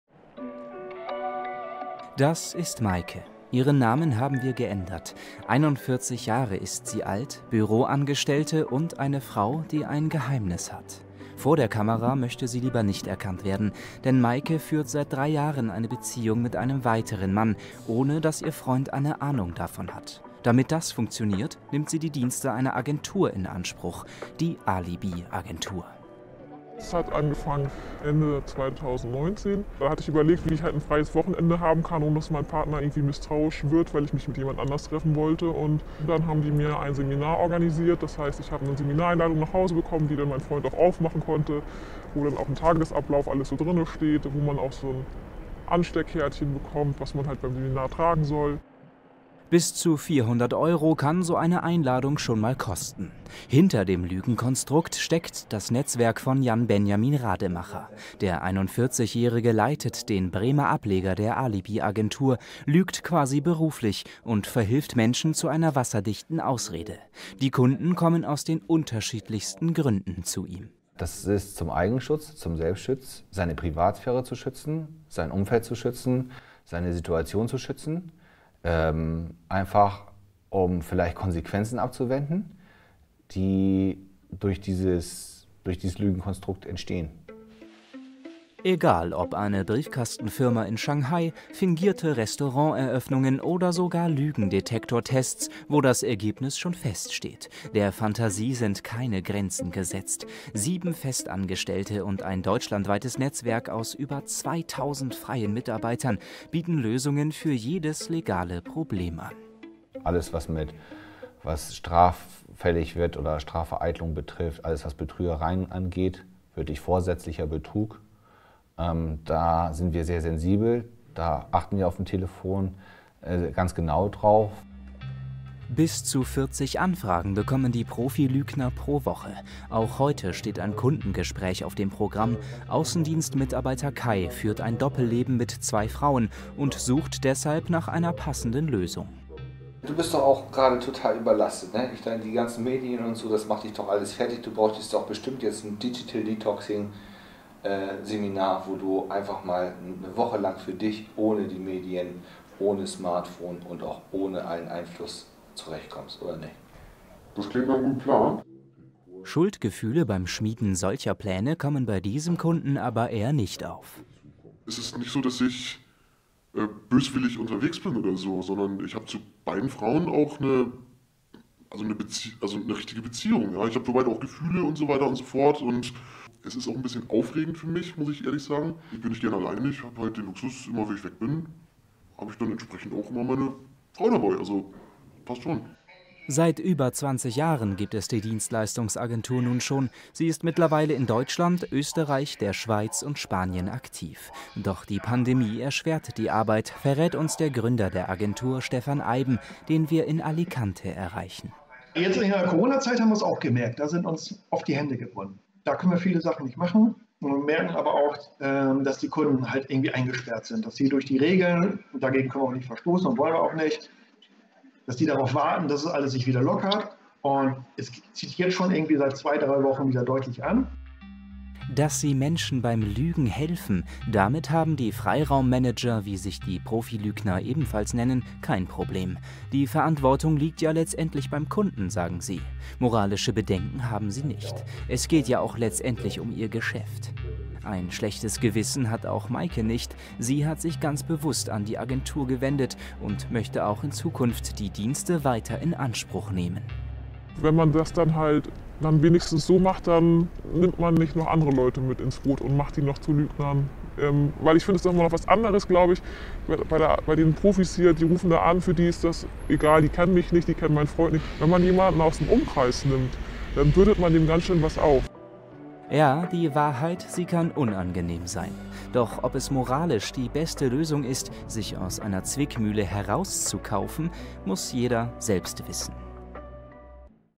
Moralisch fragwürdig: Alibi-Agentur aus Bremen bietet wasserdichte Ausrede für alle Lebenslagen
Sie wollen einen Seitensprung verschleiern oder ihren Freunden eine Postkarte aus Dubai schicken, obwohl Sie in Wahrheit eigentlich in den Harz fahren? Mit der Alibi-Agentur aus Bremen ist das kein Problem – und das schon seit über 20 Jahren. Jan-Benjamin Rademacher leitet den Bremer Ableger der Alibi-Agentur und verhilft Menschen zu einer wasserdichten Ausrede.